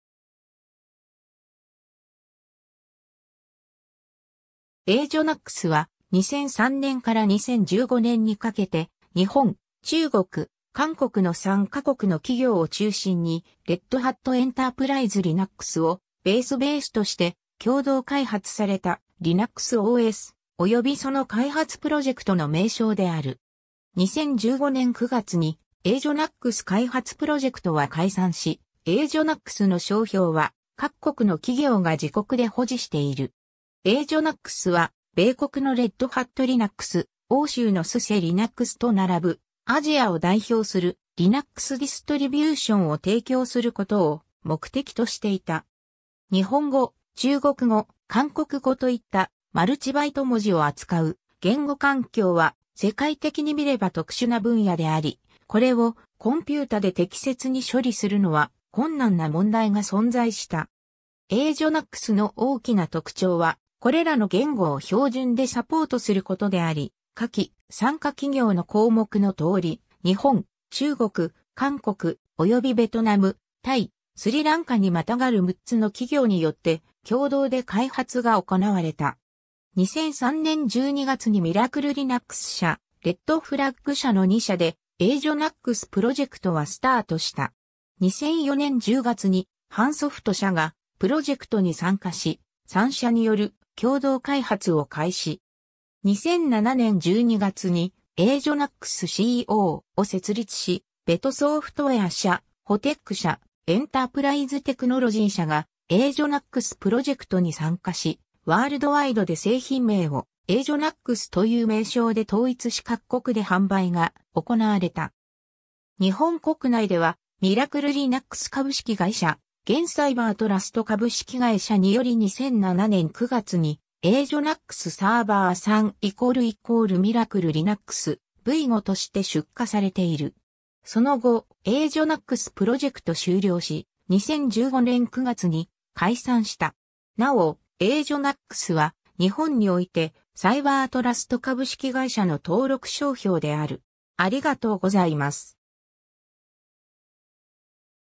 Asianux
Asianux（アジアナックス）は、2003年から2015年にかけて日本・中国・韓国の3か国の企業を中心にRed Hat Enterprise Linux (RHEL) をベースベースとして共同開発されたLinux OS、およびその開発プロジェクトの名称である。
2015年9月にAsianux開発プロジェクトは解散し、Asianuxの商標は各国の企業が自国で保持している。
 Asianux は、米国の Red Hat Linux、欧州のSUSE Linuxと並ぶ、アジアを代表するLinux ディストリビューションを提供することを目的としていた。
日本語・中国語・韓国語といったマルチバイト文字を扱う言語環境は世界的に見れば特殊な分野であり、これをコンピュータで適切に処理するのは困難な問題が存在した。
Asianuxの大きな特徴は、これらの言語を標準でサポートすることであり、下記「参加企業」の項目の通り、日本・中国・韓国、およびベトナム（2007年8月参加）、タイ（2008年12月参加）、スリランカ（2010年7月）にまたがる6つの企業によって共同で開発が行われた。
 2003年12月にミラクル・リナックス社(日本)、Red Flag社(中国)の2社でAsianuxプロジェクトはスタートした。
2004年10月にHaanSoft社(韓国)がプロジェクトに参加し、3社による共同開発を開始。
2007年12月にAsianux Co.を設立し、VietSoftware社(ベトナム)、WTEC社(タイ)、Enterprise Technology社(スリランカ)がAsianuxプロジェクトに参加し、ワールドワイドで製品名を「Asianux」という名称で統一し各国で販売が行われた。
日本国内ではミラクル・リナックス株式会社（現サイバートラスト株式会社)　により2007年9月に「Asianux Server 3 == MIRACLE LINUX V5」として出荷されている。その後、Asianuxプロジェクト終了し、2015年9月に解散した。なお、Asianuxは日本においてサイバートラスト株式会社の登録商標である。